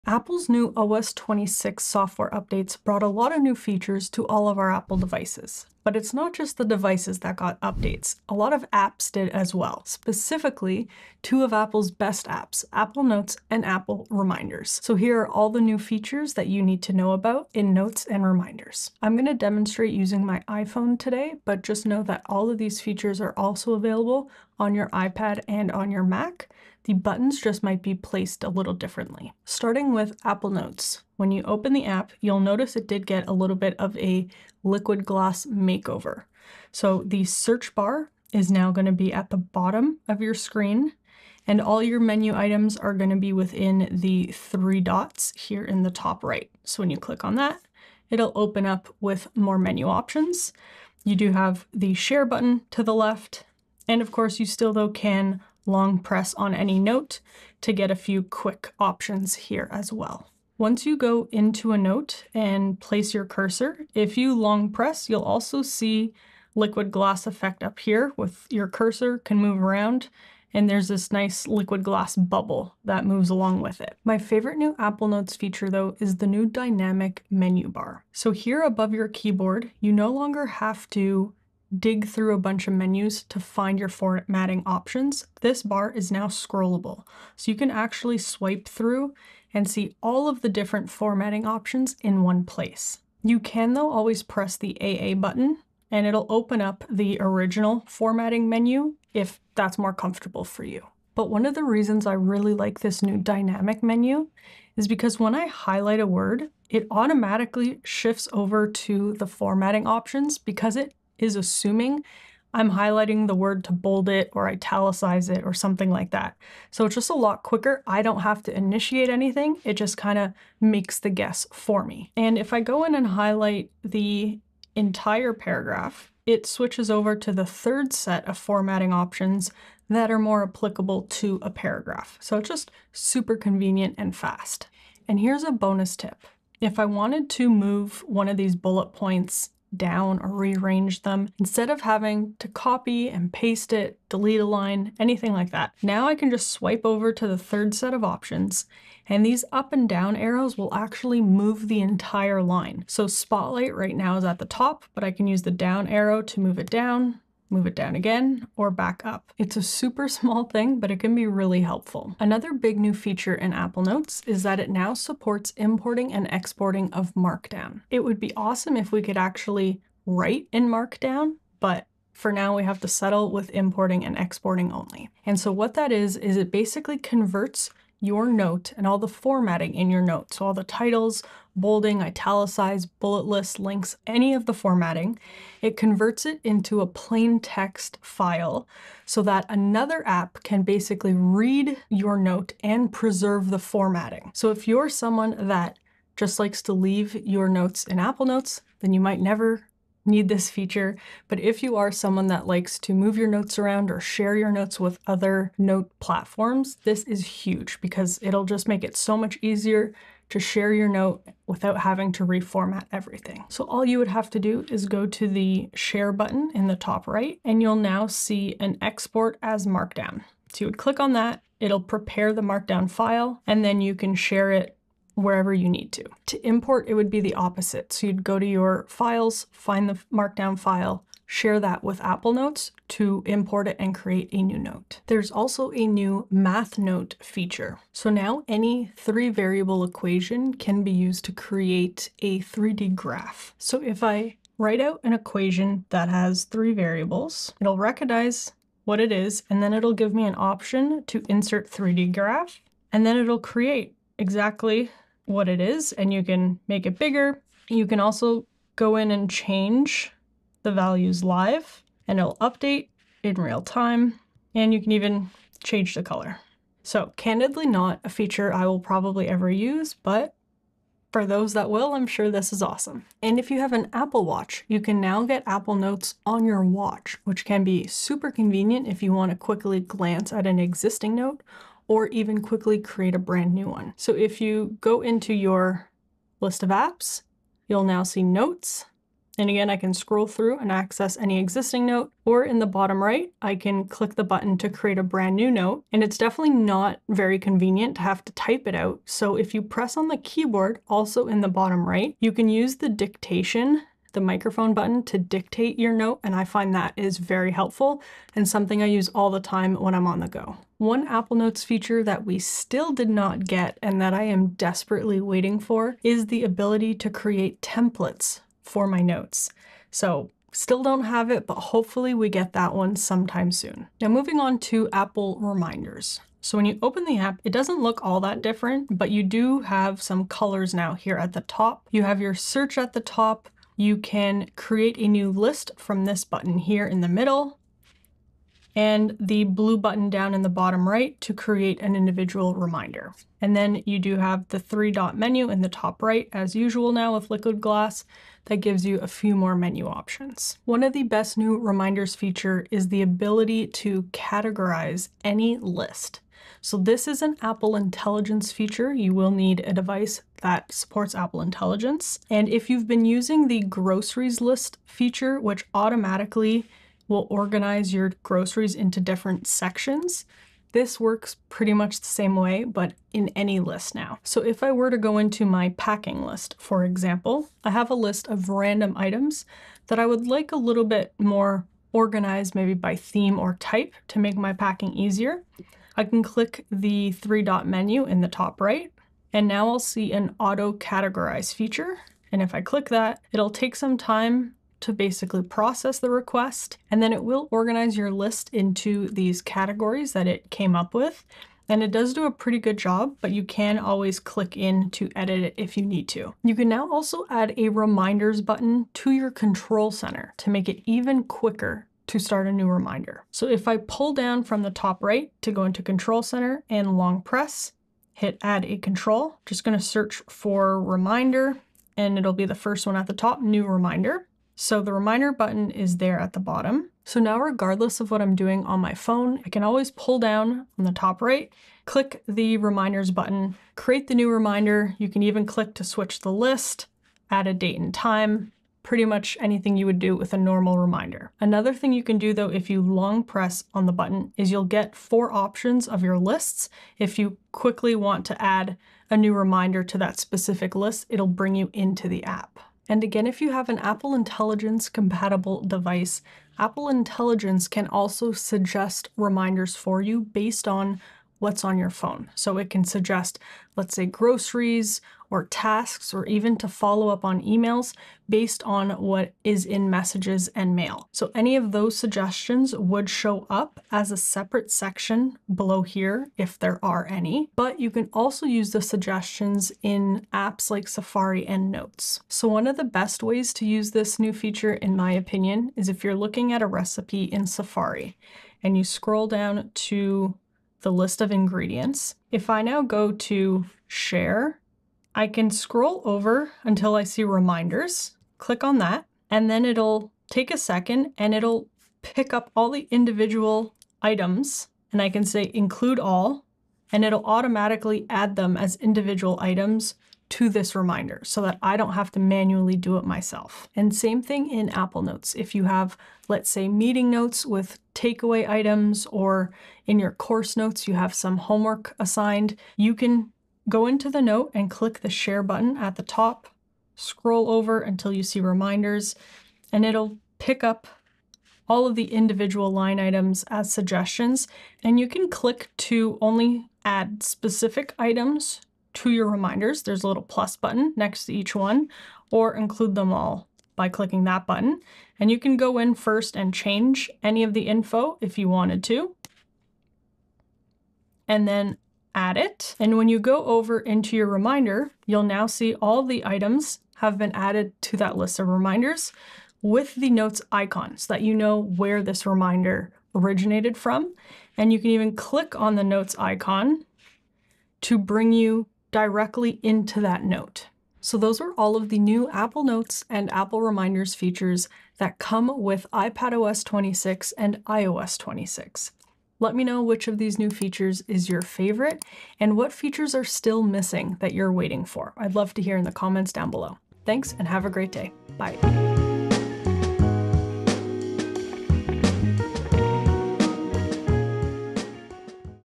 7 NEW Apple Notes and Reminders Features You'll Want to See!
✨ Apple just dropped some awesome updates to the Notes and Reminders apps in iOS & iPadOS 26, and some are way better than you’d expect! We’ve got a new Dynamic Menu, Markdown export, Math Notes, and smart Apple Intelligence suggestions that can even pull tasks straight from your messages or emails.

0:00 OS26
0:29 notes
1:14 dynamic menu
2:44 markdown
3:55 math notes
4:37 apple watch
5:14 missing feature
5:23 reminders
5:53 auto-categorize
6:46 control centre
7:35 ai suggestions
9:31 what's missing